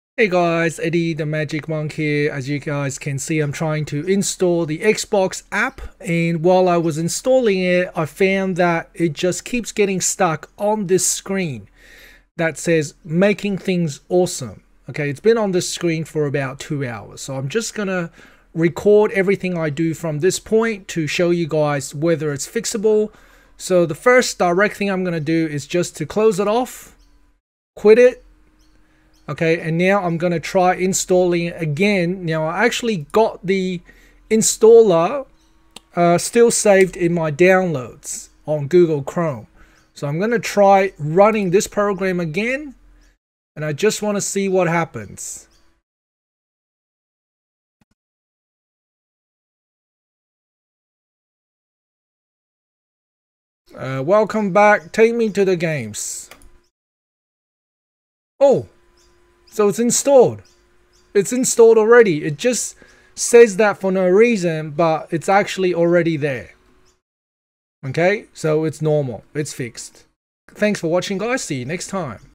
Xbox installer stuck on Making Things Awesome
So I signed up to Xbox Game Pass Ultimate and tried to install the Xbox desktop app for Windows PC. It seemed to be stuck on a screen with the writing "Making Things Awesome". It certainly took a while to make things awesome - I went for breakfast and watched a whole movie, came back and it was still saying "Making things awesome". This is what I did to get into the Xbox app finally..